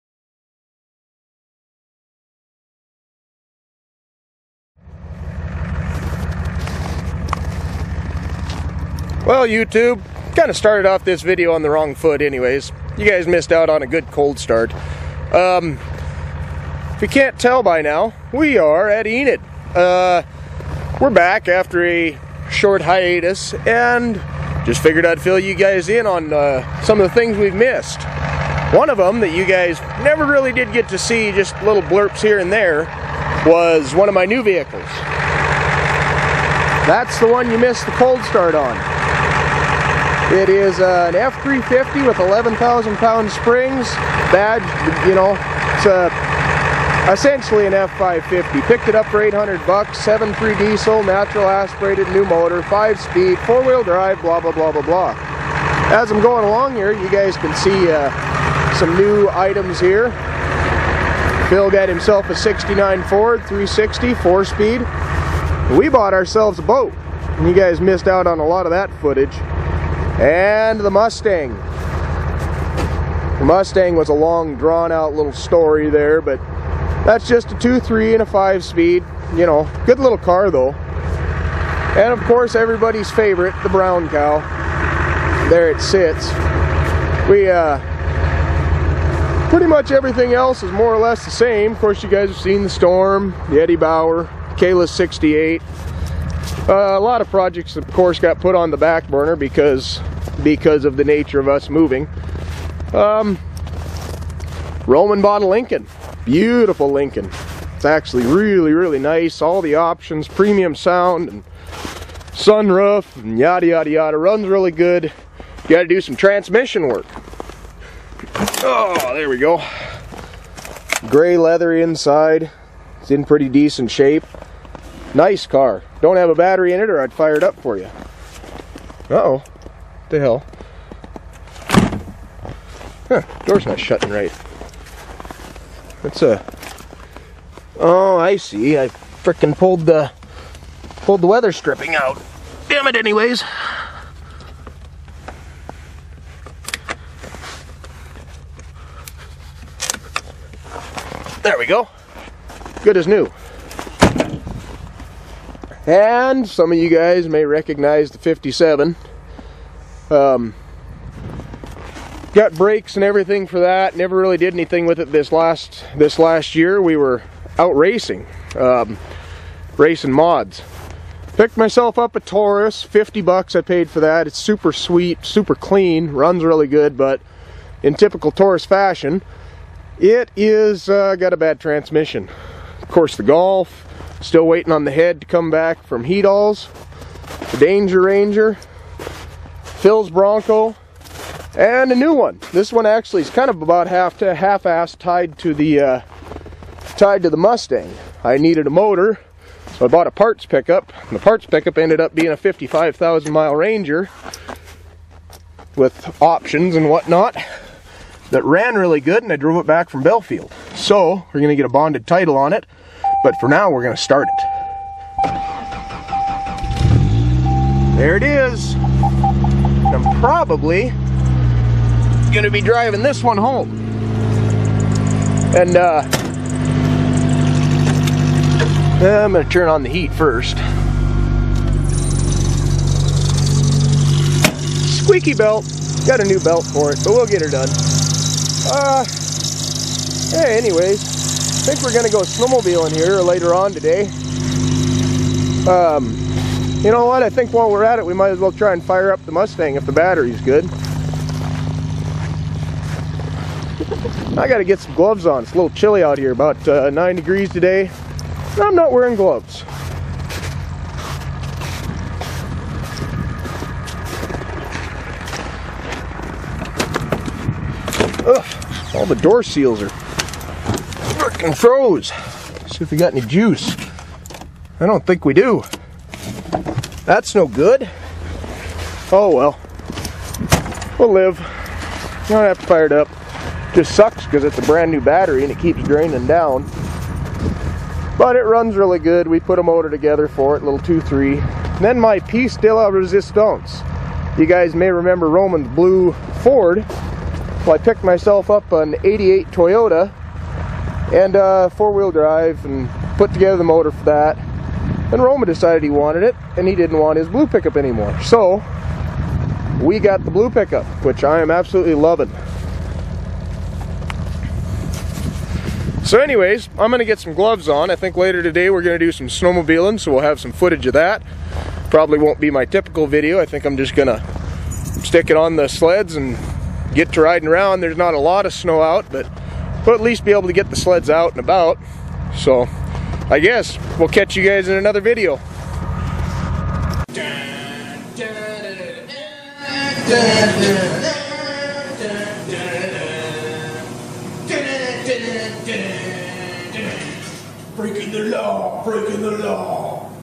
Updates out at Enid with Cold Start!
We show you some of the vehicles out at Enid and give you a nice 9 above cold start.